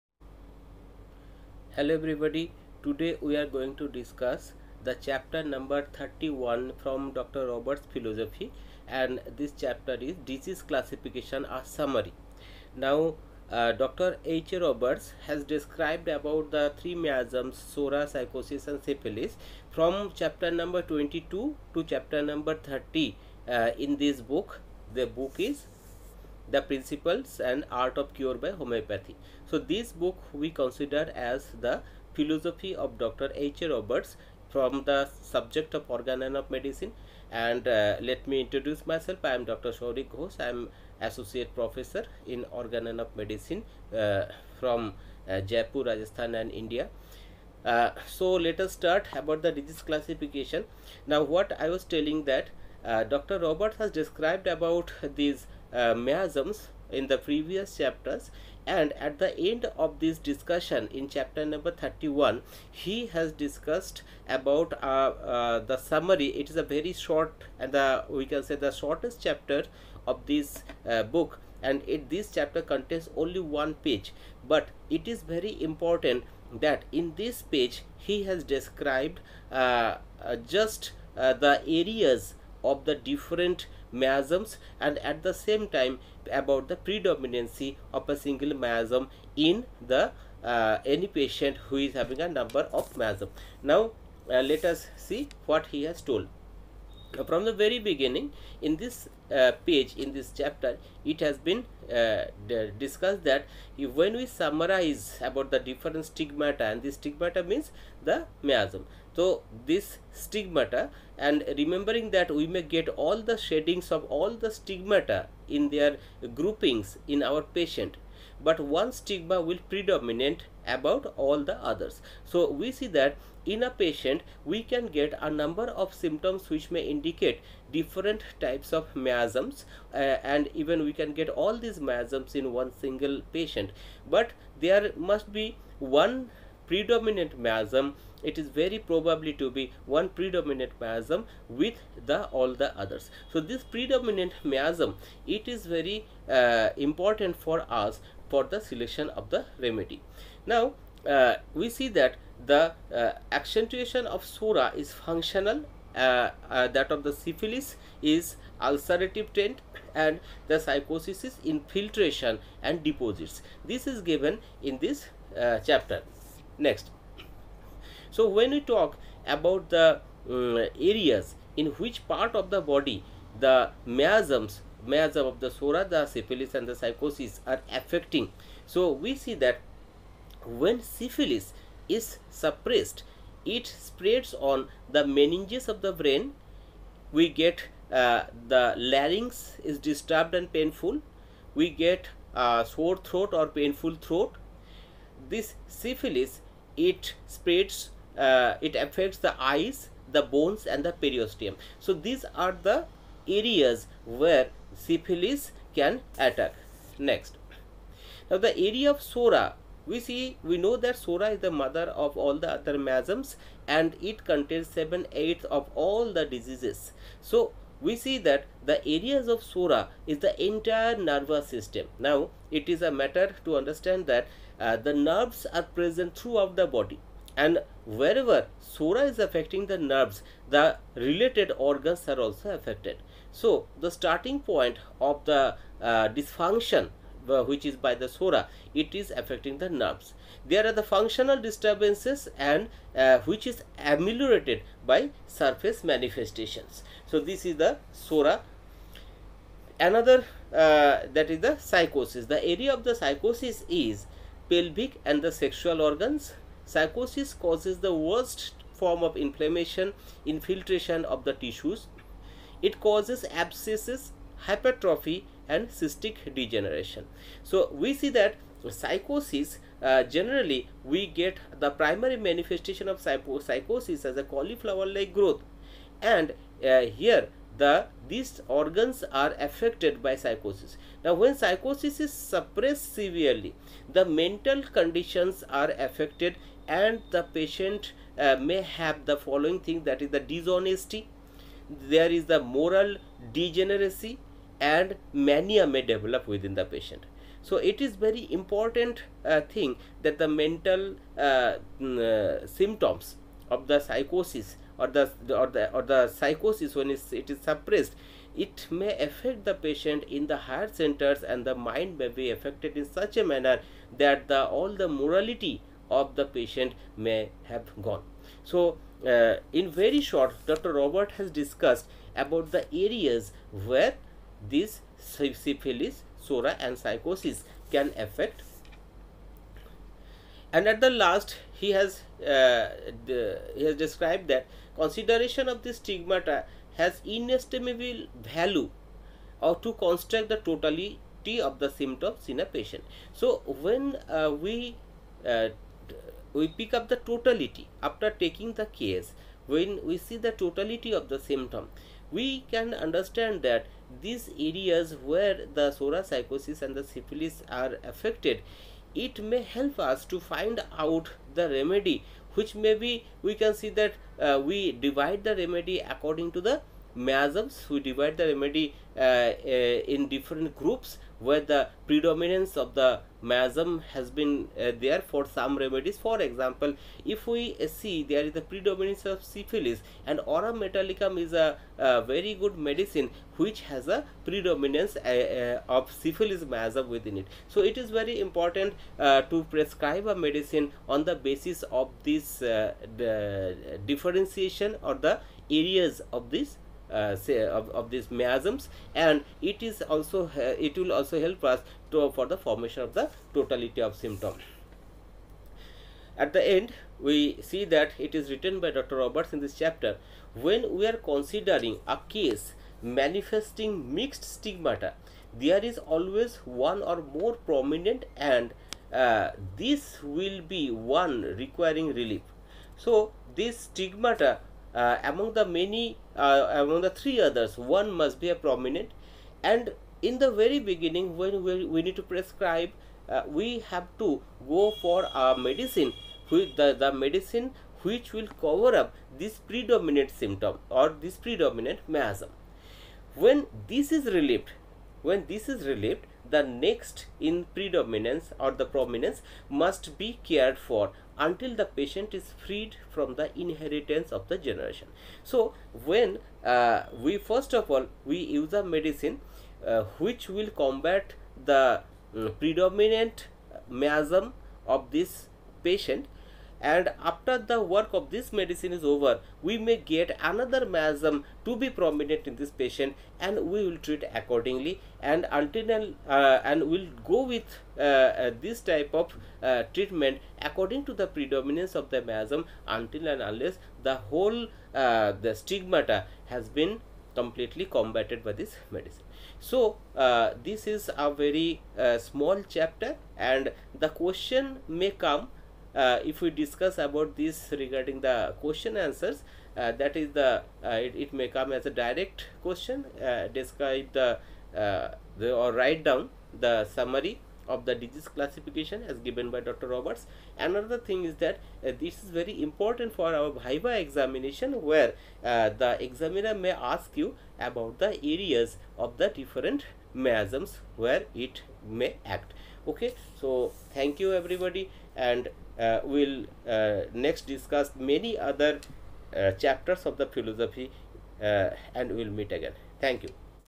Robert's philosophy Chapter 31 Disease classification-a summary
Robert's philosophy chapter 31 Disease Classification-a summary, described in details. Please note the site of affections of the miasms